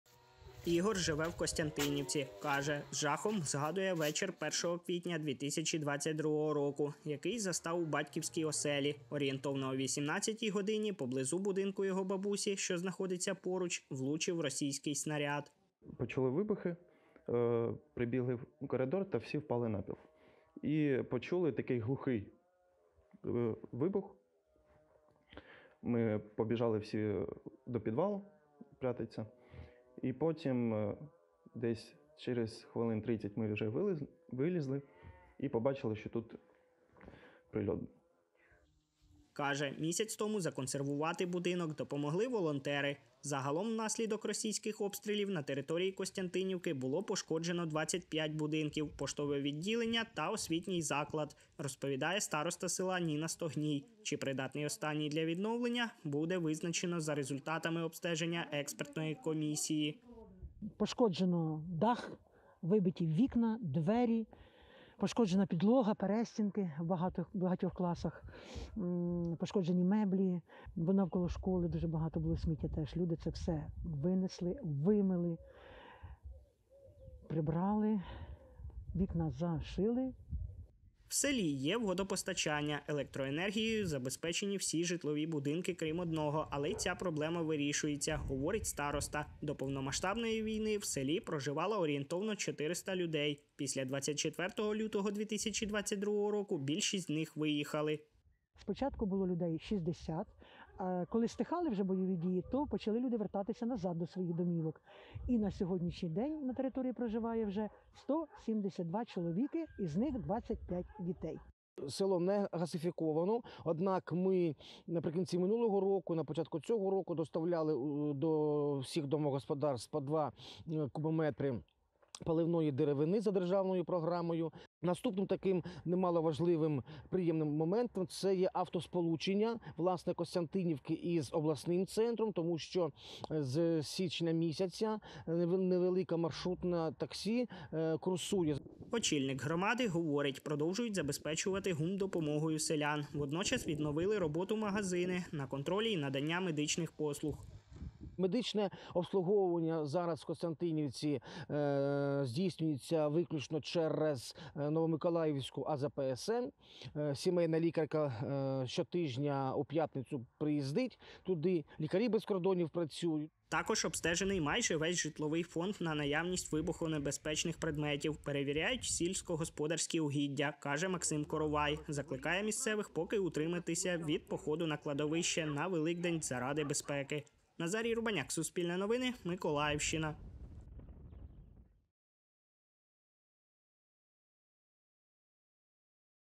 Костянтинівка за 5 місяців після обстрілів. Як відновлюється колишнє прифронтове село
Село Костянтинівка Первомайської селищної громади було прифронтовим з початку березня до 10 листопада 2022 року. За цей час пошкоджень і руйнувань зазнали житлові будинки та об'єкти інфраструктури, розповіла староста Ніна Стогній.